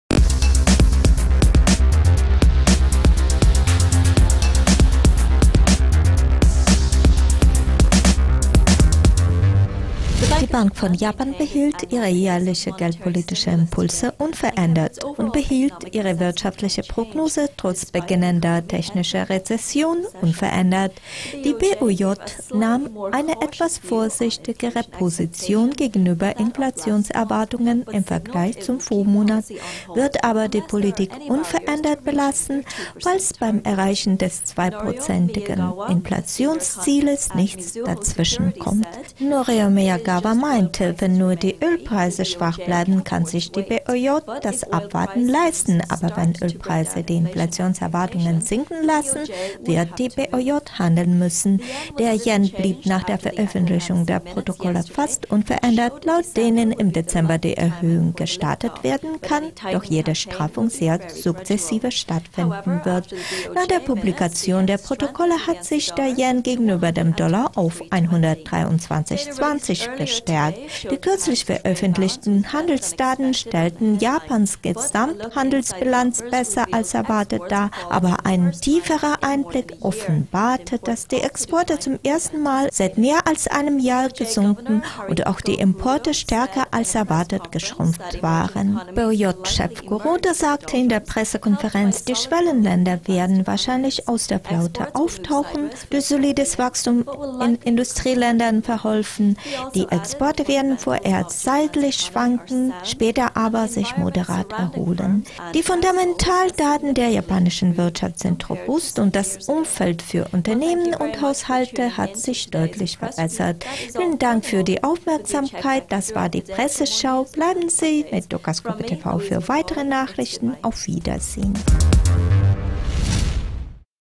BoJ-Politik bleibt unverändert
Trotz beginnender technischer Rezession bleibt die Geldpolitik der BoJ unverändert.

Sie können dieses Video und das gesamte Video Archive auf der Dukascopy TV Seite ansehen